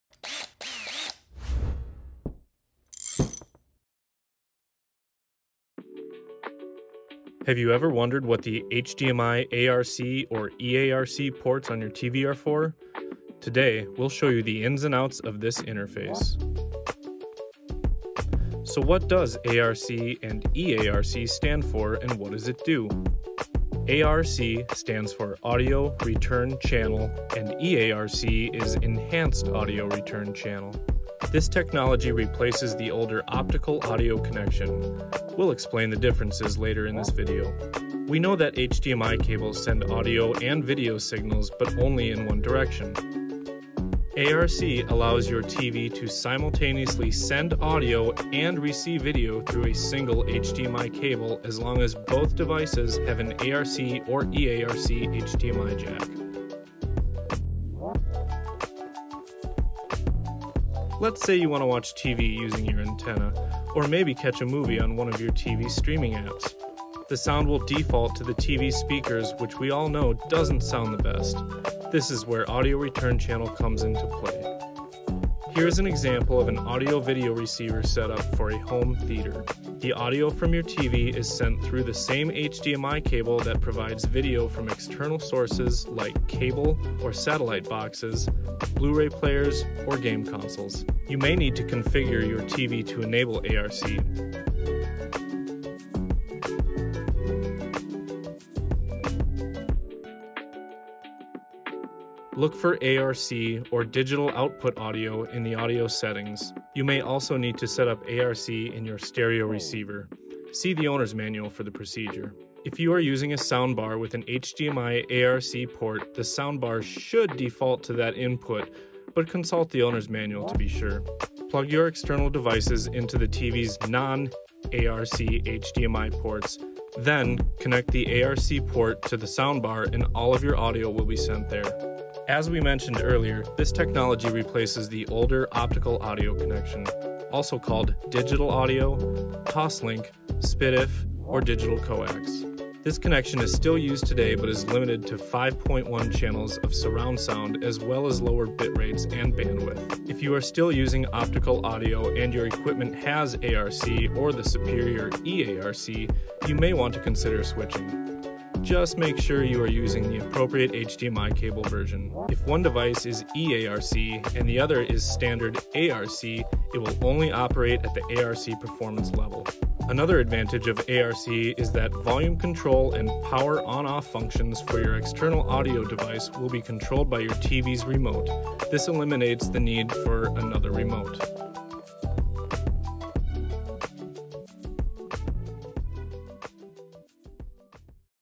Understanding Different HDMI Ports in LED TVs - HDMI (ARC) vs. HDMI (eARC) - TV Tech Series
We delve into HDMI ARC (Audio Return Channel) and eARC (Enhanced Audio Return Channel), explaining their significance and functionality in transmitting audio and video signals through a single HDMI cable. We also guide you through configuring your TV and audio device, demonstrate how ARC improves your home theater experience, and discuss the limitations of older optical audio connections.